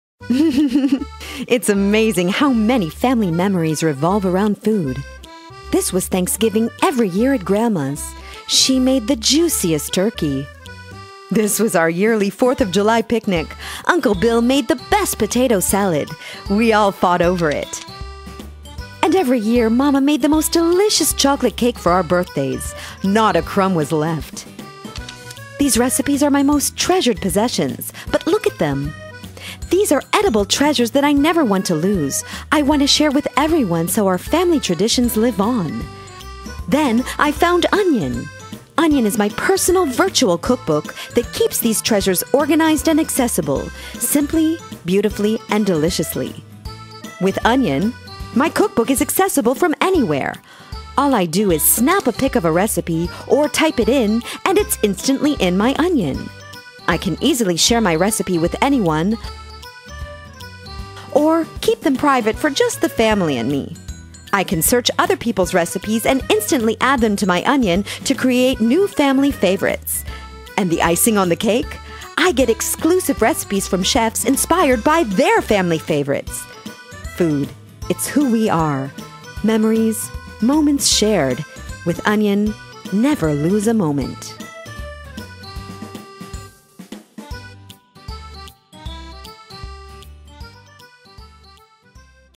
Onion
A product video for Onion's App - the app to save, create and share food recopies. produced and created by Visual3D